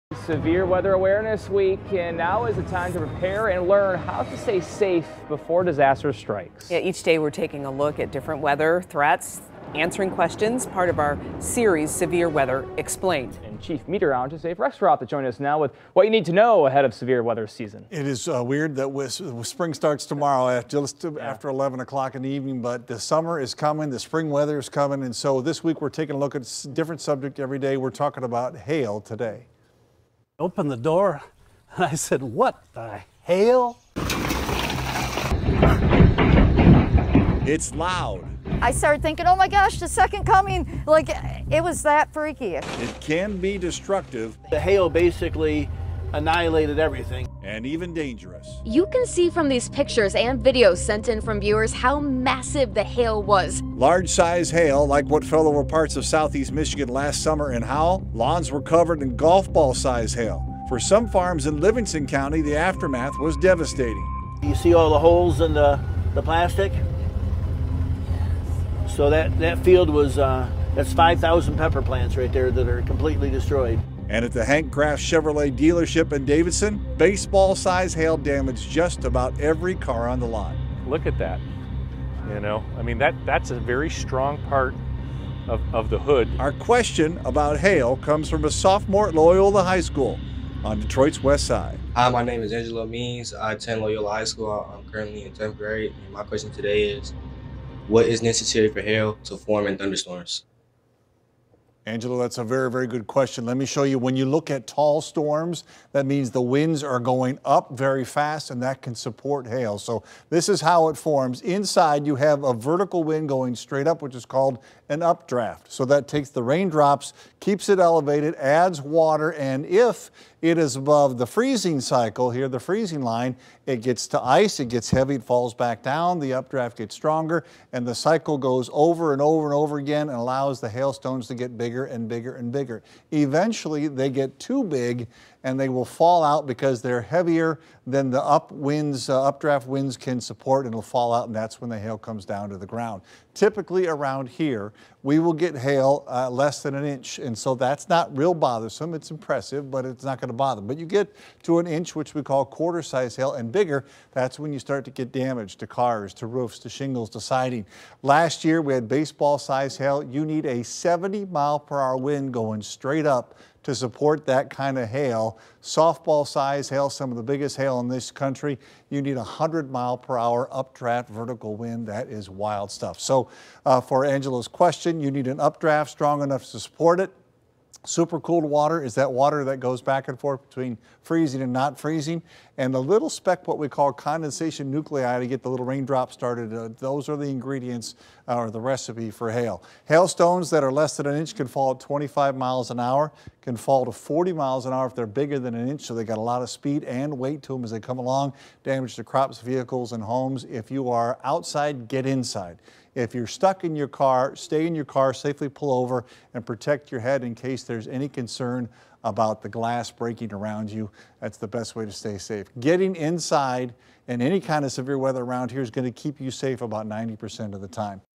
Severe weather explained: Hail
It is Severe Weather Awareness Week. We are taking a look at a different weather threat -and answering questions as part of our series - Severe Weather Explained.